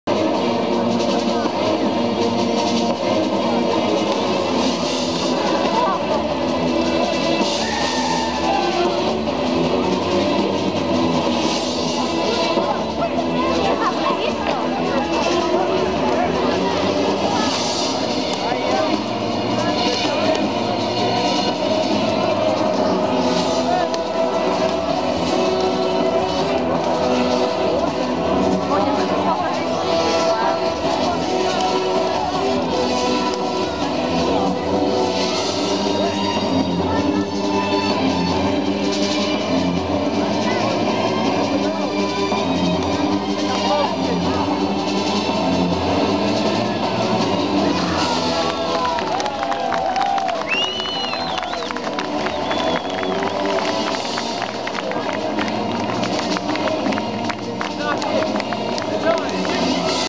Un momento della "Lotta per la spada dei Contrari" (Parte 2)
Un momento dello spettacolo di combattimento fra cavallieri alla festa medioevale di Savignano (Modena) chiamata "Lotta per la spada dei Contrari"